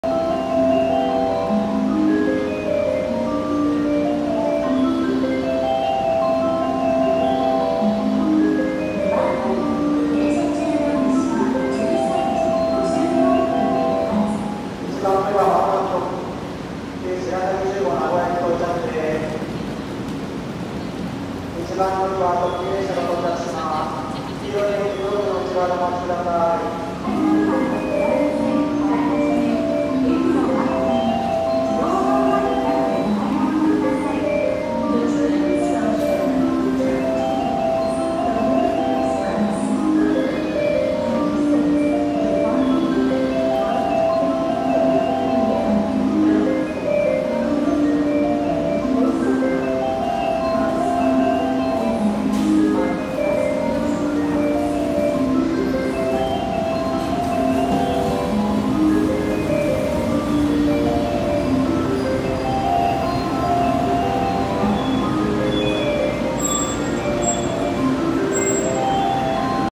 特急しらさぎ 金沢駅到着
先日に引き続き金沢駅の動画です。
特急しらさぎを撮影しました。あと少しで金沢駅から消えてしまいますが、寂しいですね。